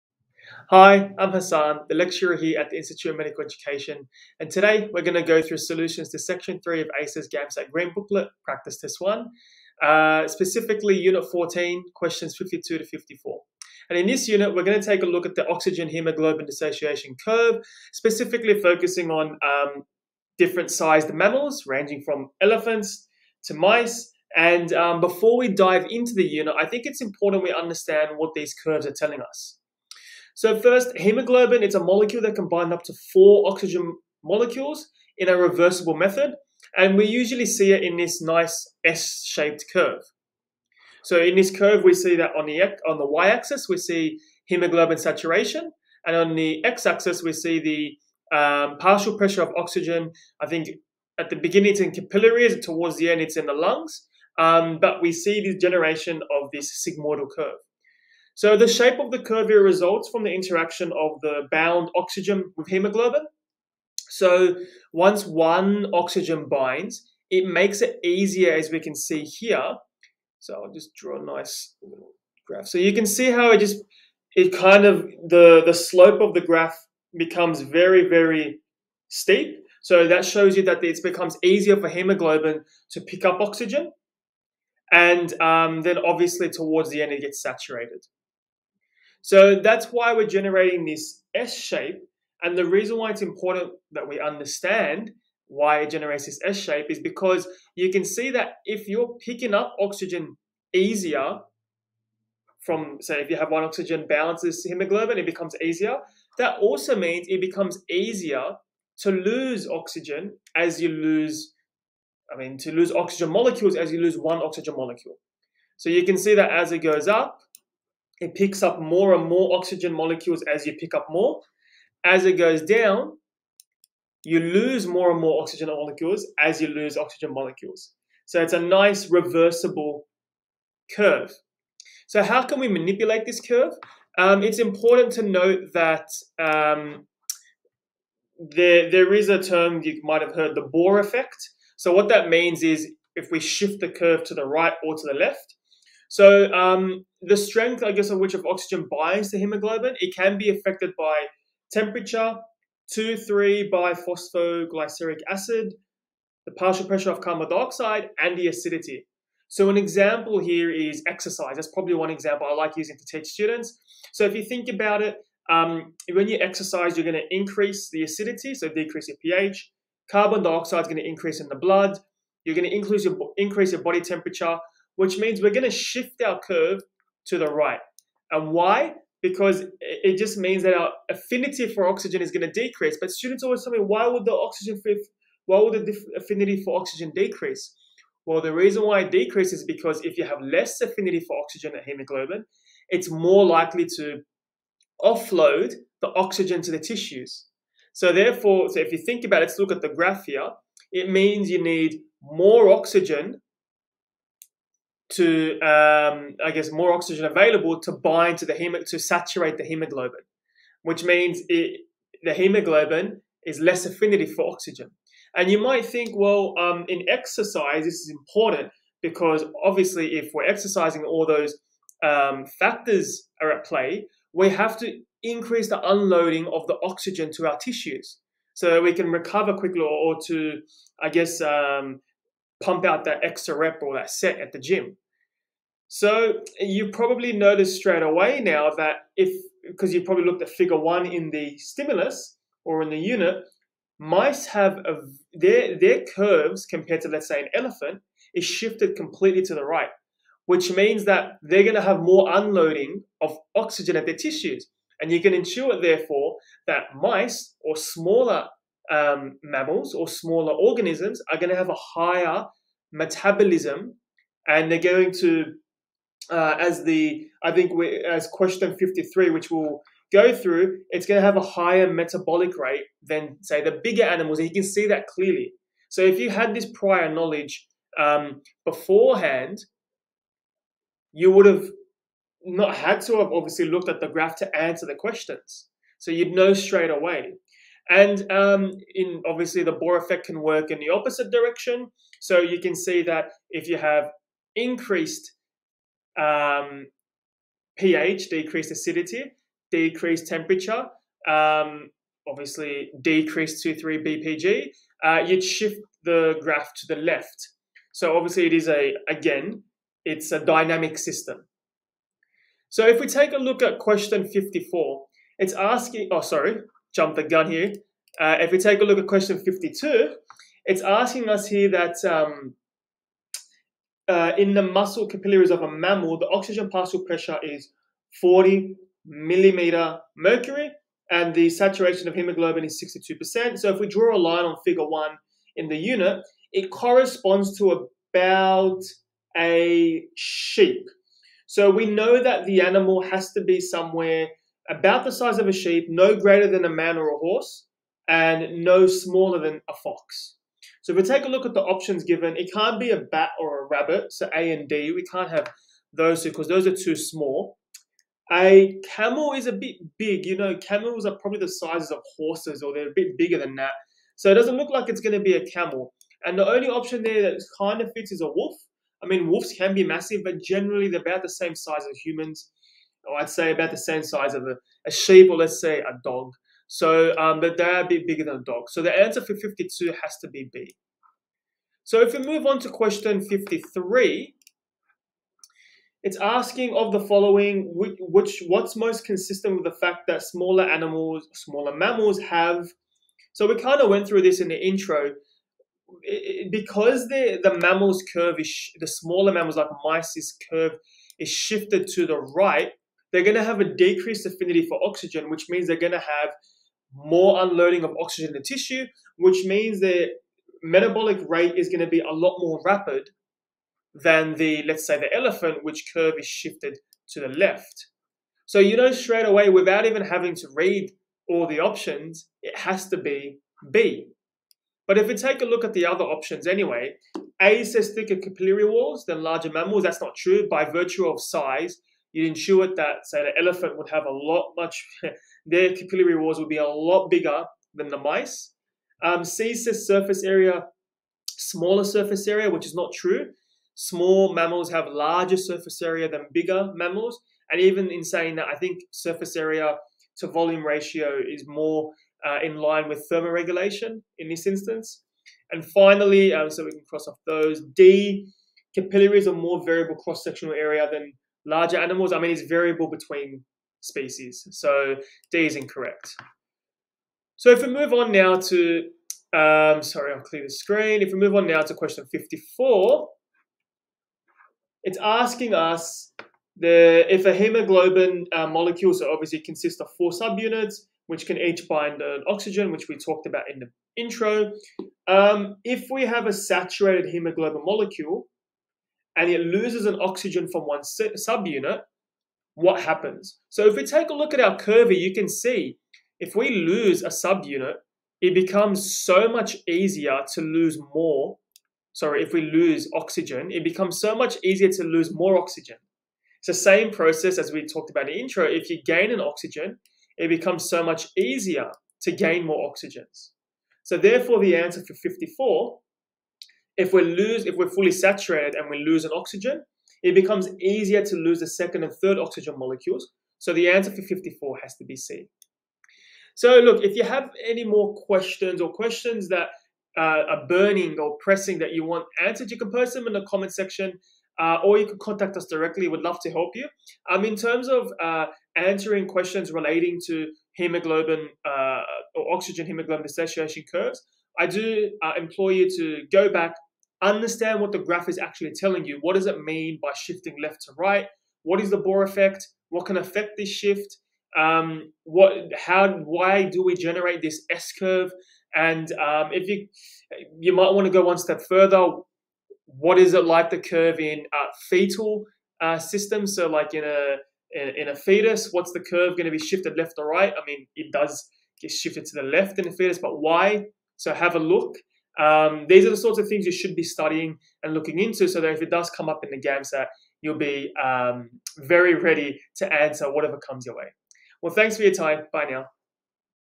ACER GAMSAT Solutions to Unit 14: Question 52–54 (Practice Test 1, formerly Green Booklet)
Our lecturer Hassan will go through the above ACER Practice Test 1 questions with you, formerly known as Green Booklet.

Facebook: Institute of Medical Education – IME
LinkedIn: Institute of Medical Education – IME